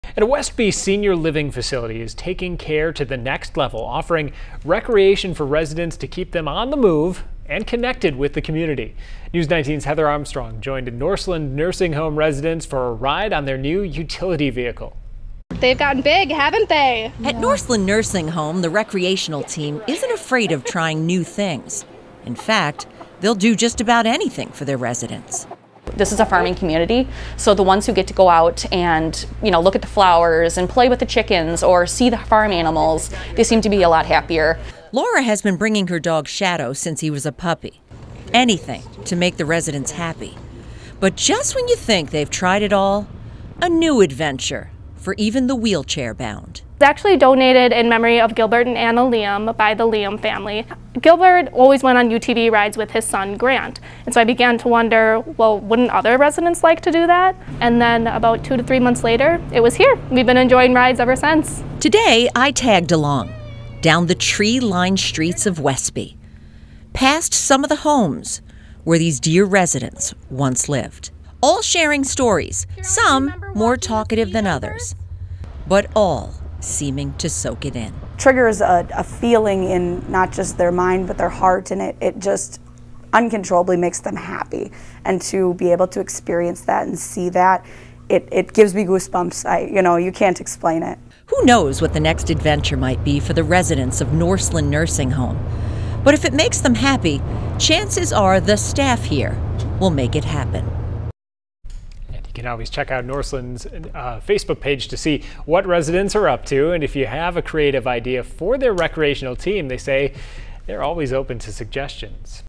Norseland Nursing Home residents take advantage of new UTV
A Westby senior living facility is taking their care to the next level by keeping residents on the move.